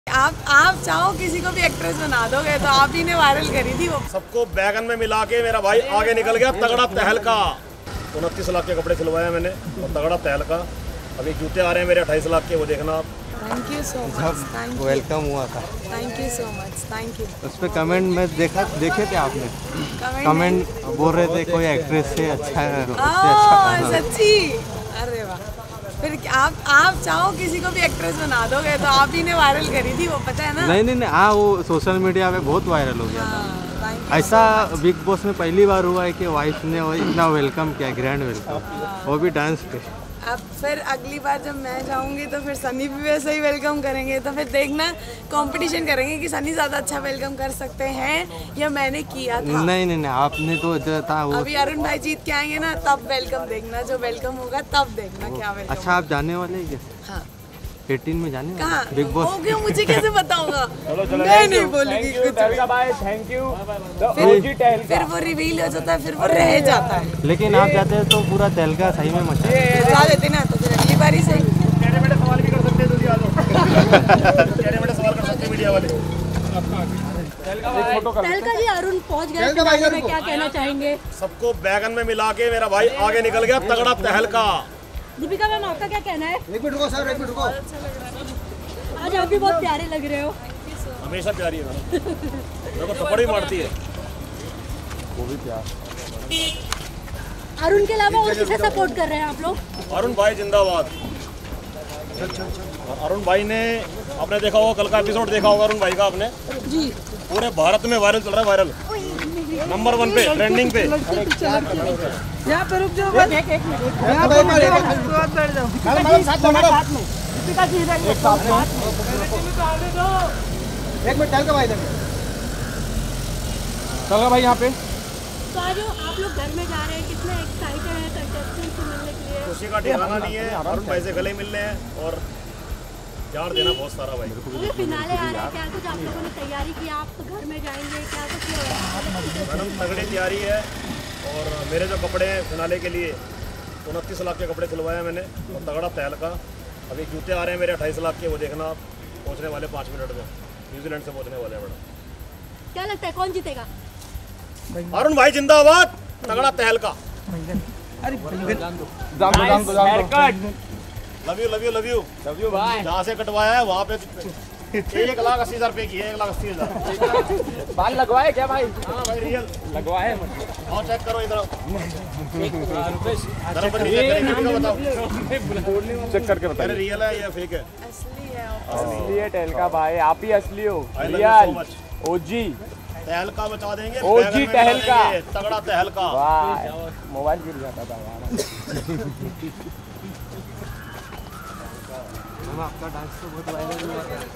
BB17: Tehelka की Wife Deepika Arya जाएंगी Abhishek, Arun, Ankita से मिलने, मजेदार Video हुआ Viral!
BB17: Tehelka's wife Deepika Arya spotted outside the Bigg Boss set, Talks about Ankita, Abhishek & Arun. Watch video to know more